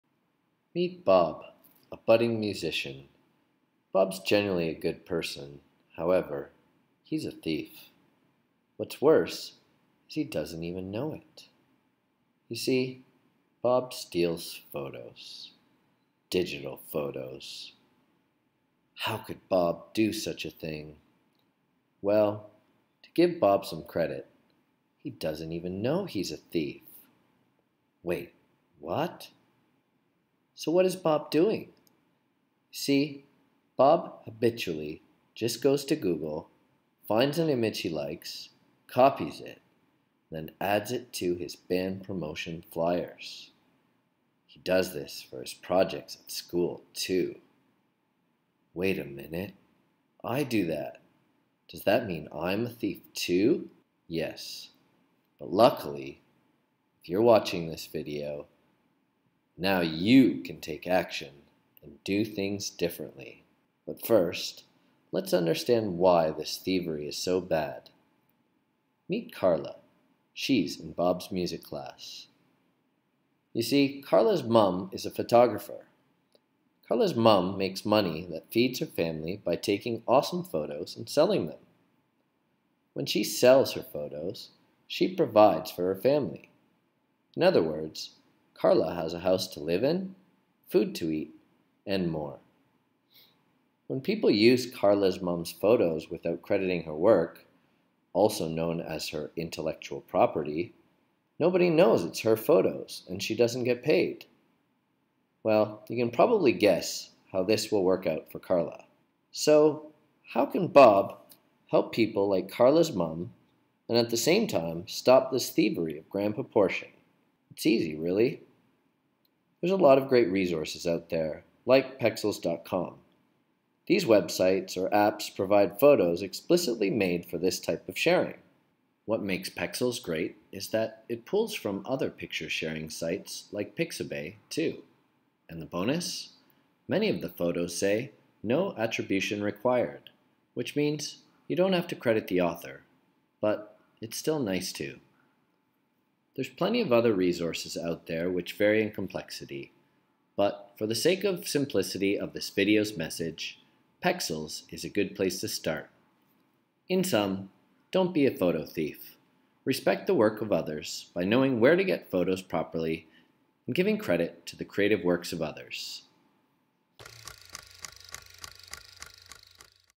Digital citizenship for using online photos
Do you know how to source photos online whilst respecting the work of others? If you're unsure, then watch this video!

Give it a 👍 if you found it helpful 😁